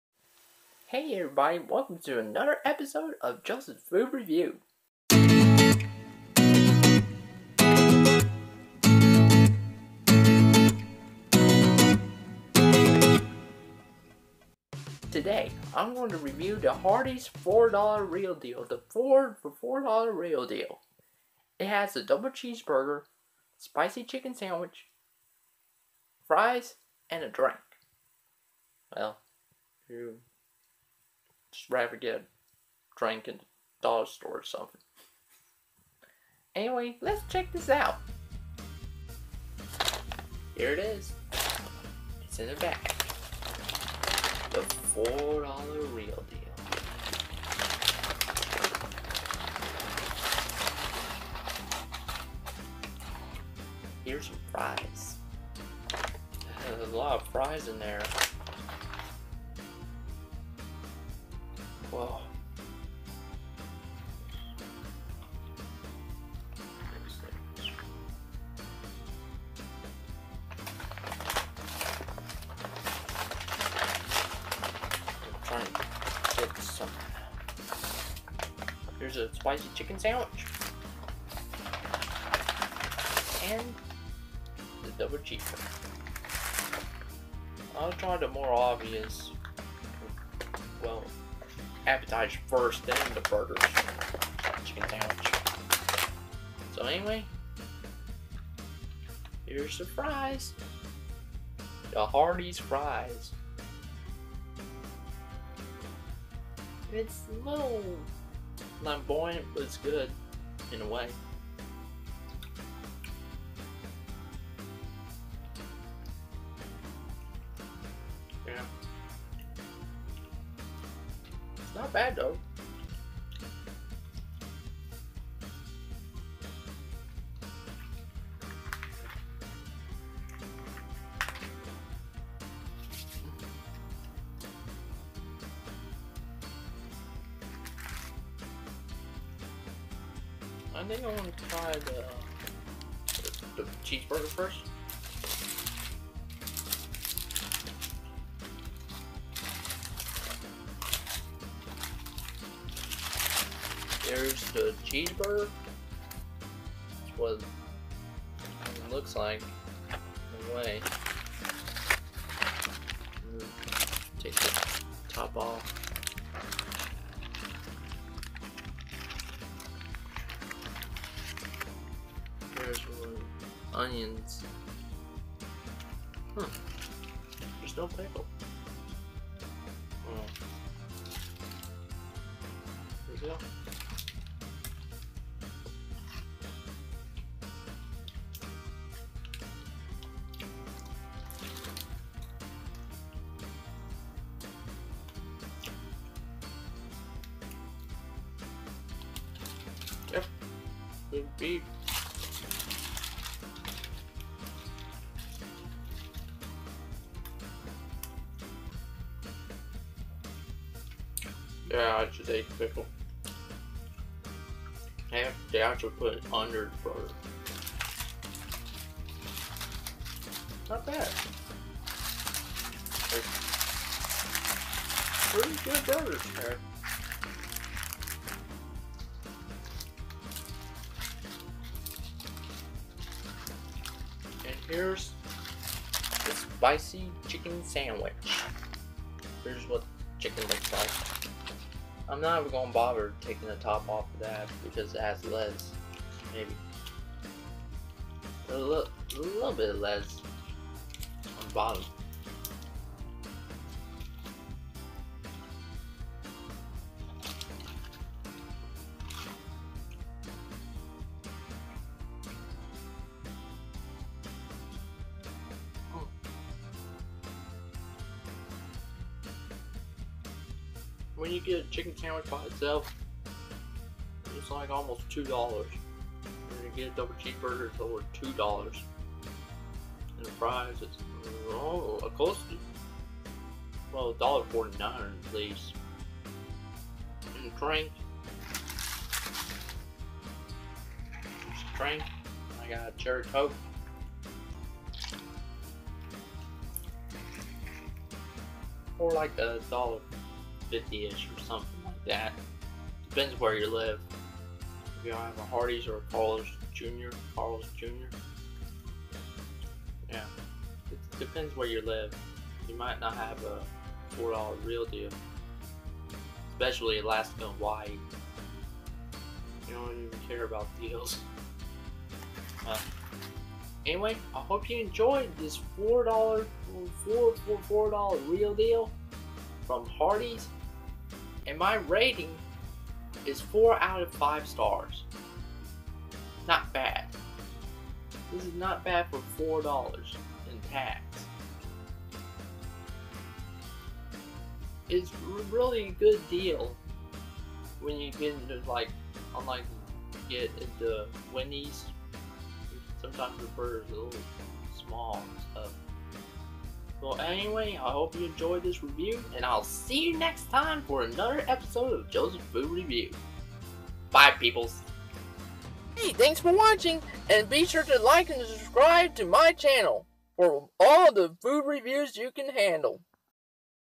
JFR Season 3 Episode 2- Hardee's 4 for $4 Real Deal
My review of Hardee's 4 for $4 Real Deal